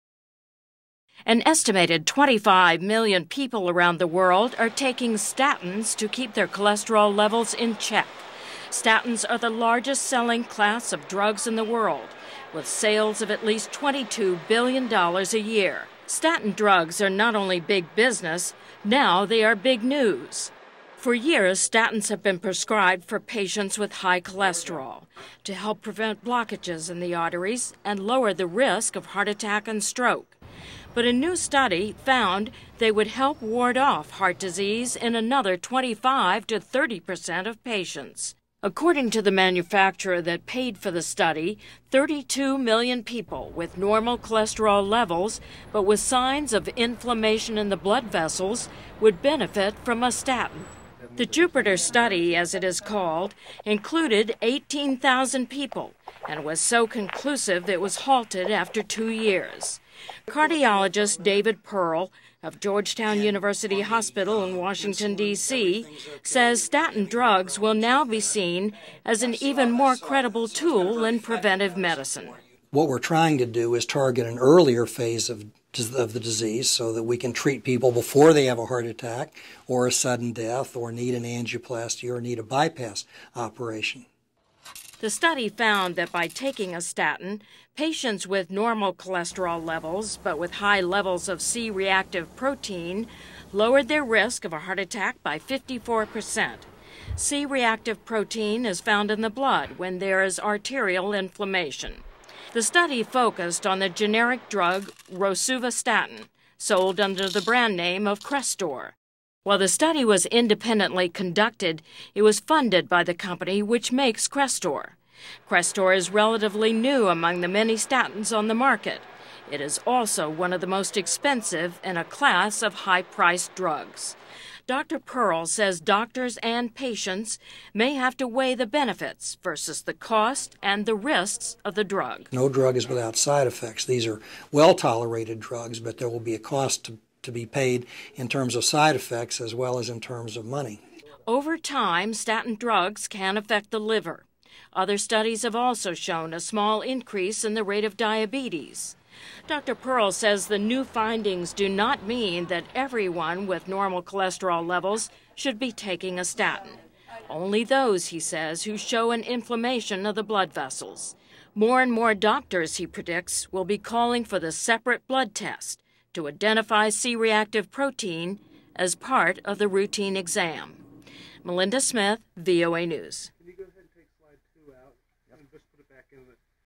Statin Study: More Patients at Risk for Heart Disease
Doctors say a new study by the makers of the statin drug Crestor may help identify more patients with hidden risk of heart attack and stroke.  The study released at a meeting of the American Heart Association Sunday [November 9th] suggests that even people with normal or low cholesterol levels stand to benefit from taking a cholesterol lowering statin drug when a separate blood test identifies high levels of arterial inflammation.